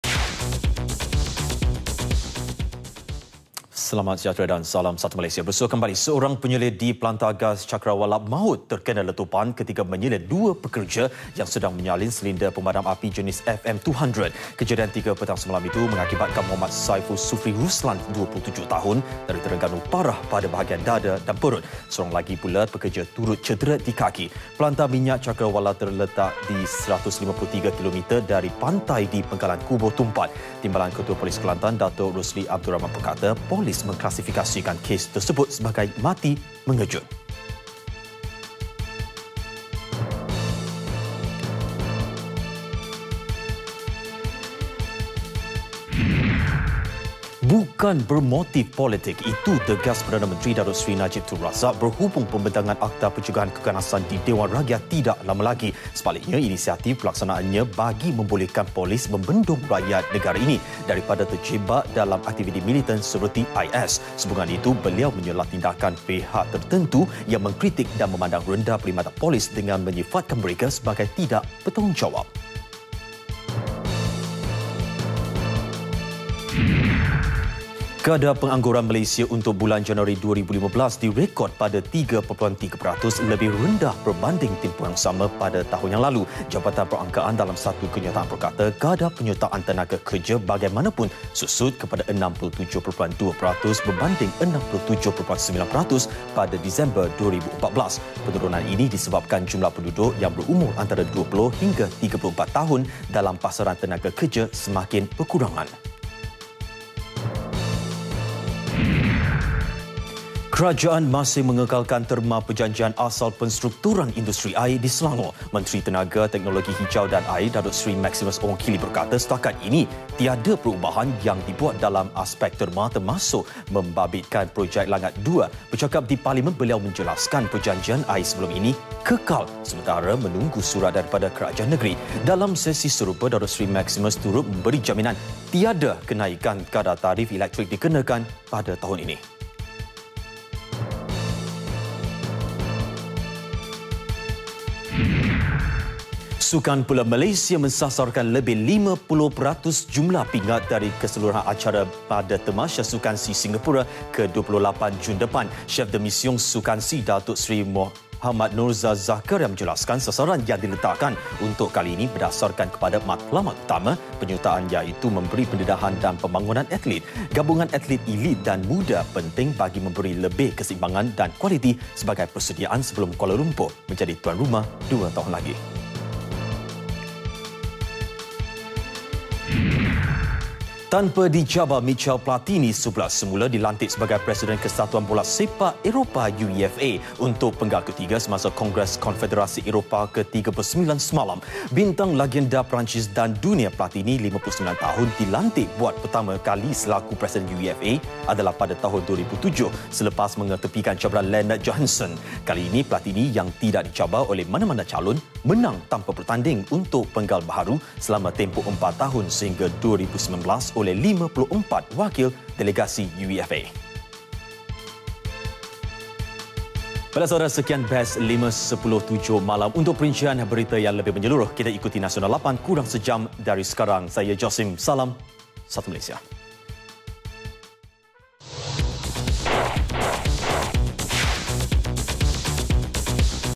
BES 5-10 7 PM [25 MAC 2015]
1. Seorang penyelia di Pelantar Gas Cakerawala MAUT.
2. Bukan bermotif politik - PM.
3. Kadar pengangguran Malaysia untuk bulan Januari 2015 direkod pada 3.3 peratus, rendah.
4. Kerajaan masih mengekalkan terma perjanjian asal penstrukturan industri air di Selangor.
5. Malaysia sasar lebih 50% jumlah pingat pada temasya sukan Sea Singapura ke-28.
6. Michel Platini sebulat semula dilantik sebagai Presiden UEFA.